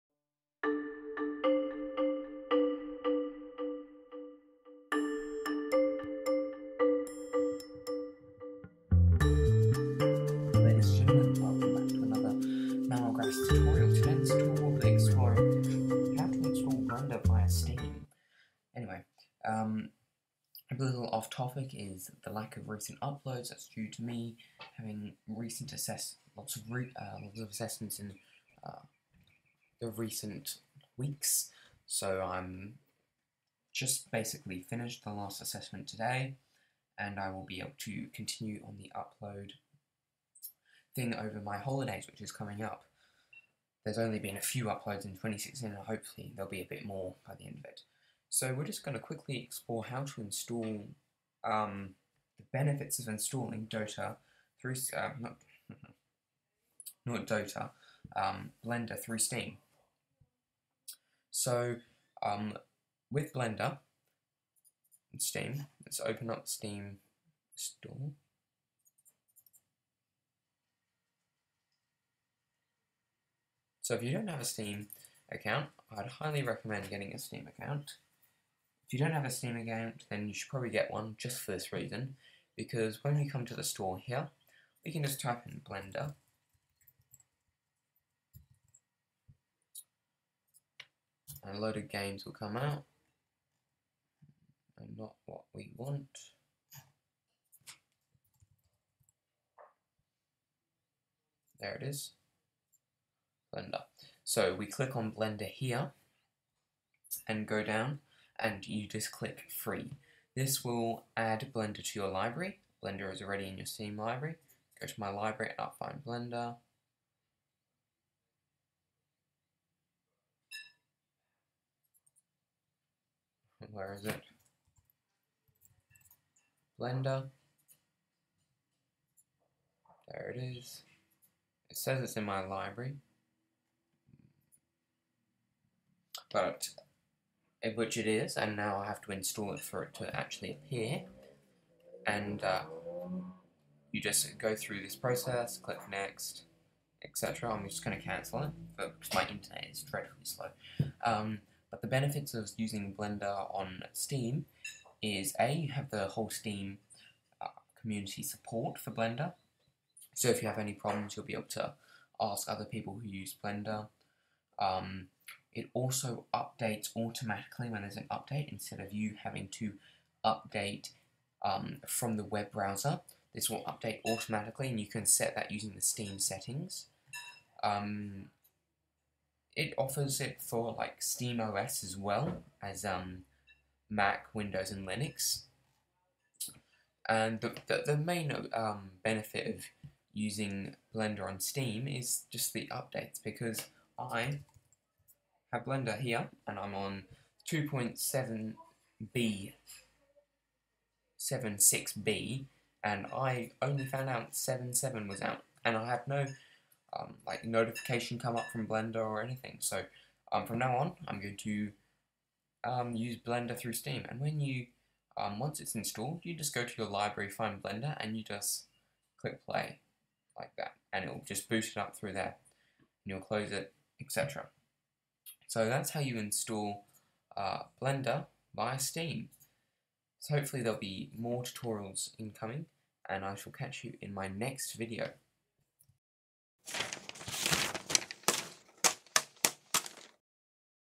Tutorial: Install Blender via Steam
Hello and welcome, to a brand new video. In this video I will go over how to install Blender via Steam. With this way, Blender will be constantly updated without you having to access the website.

-Manuel Graphics